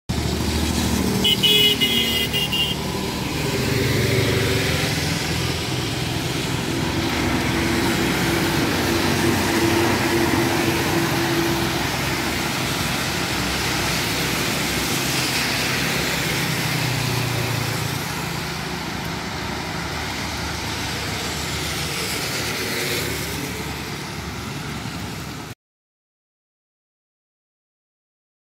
GSRTC"Shetrunjay"Sleeper Coach in Heavy Rain.
GSRTC"Shetrunjay"Sleeper Coach Bhavnagar to Mandvi in Heavy Rain.